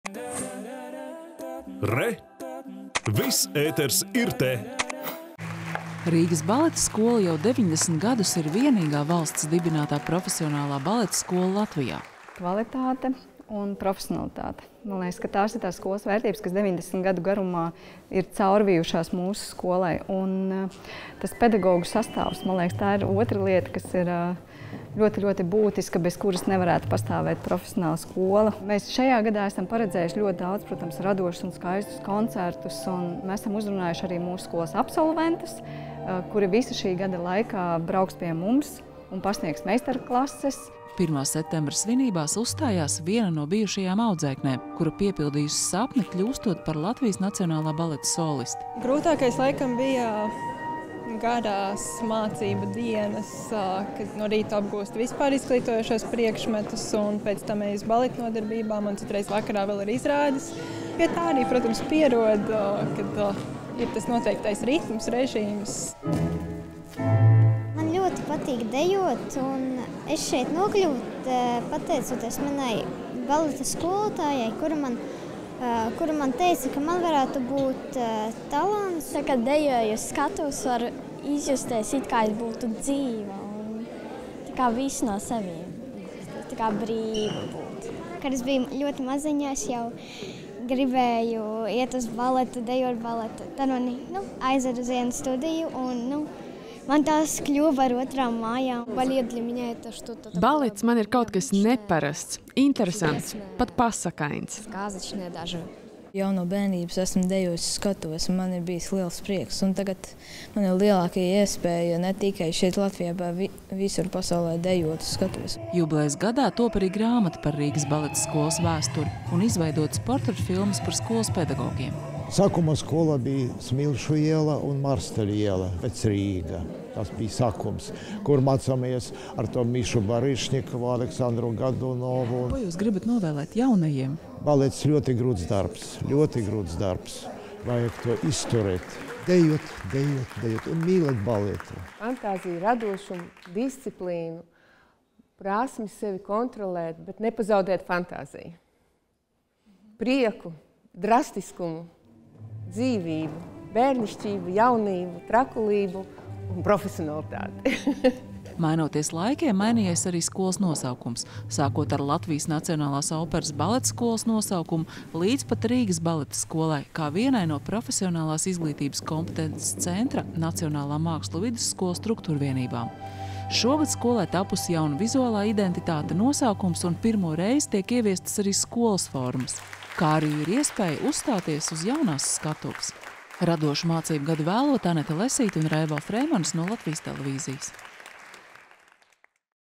Rīgas Baleta skola uzsāk 90.jubilejas mācību sezonu
Šodien, 1. septembrī, uzsākot jaunu, radošu mācību gadu,  svētku gaisotnē paviesojāmies “Rīgas Baleta skolā”, kas šajā gadā svinēs 90. jubileju.  Zinību dienas noskaņu tvēra Anete Lesīte.